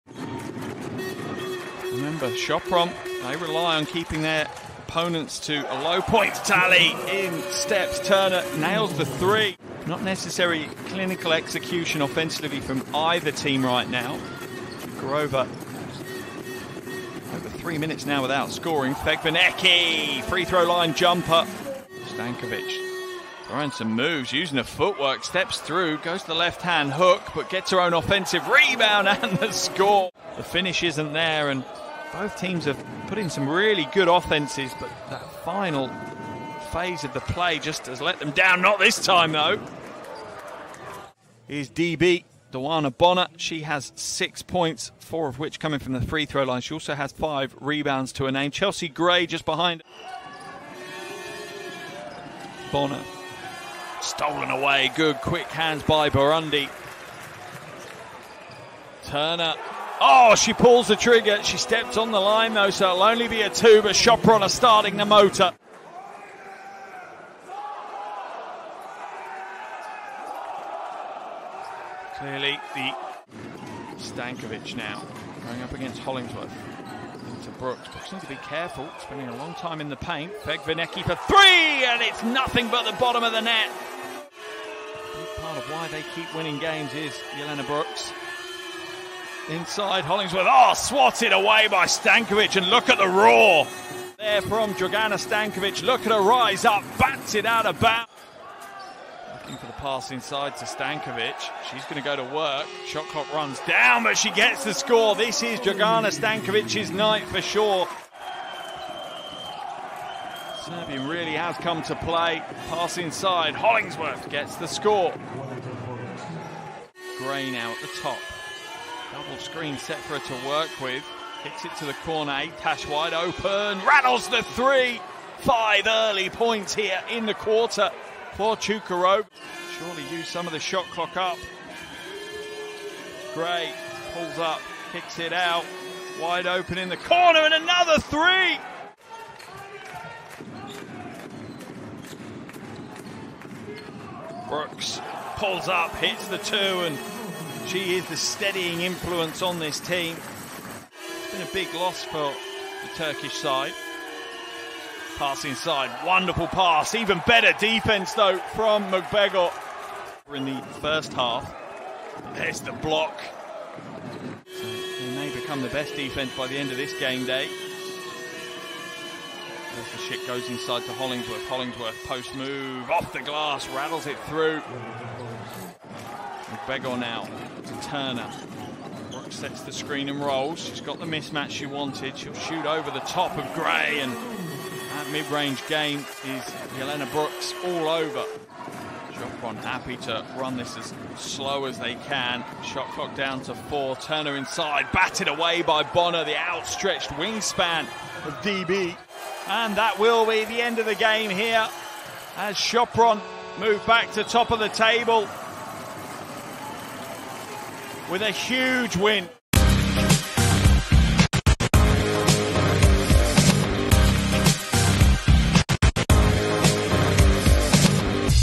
Sopron Basket v CBK Mersin Yenisehir Bld | Gameday 9 | Highlights | EuroLeague Women 2022-23
Enjoy the highlights of the game between Sopron Basket and CBK Mersin Yenisehir Bld from Gameday 9 of the EuroLeague Women 2022/23!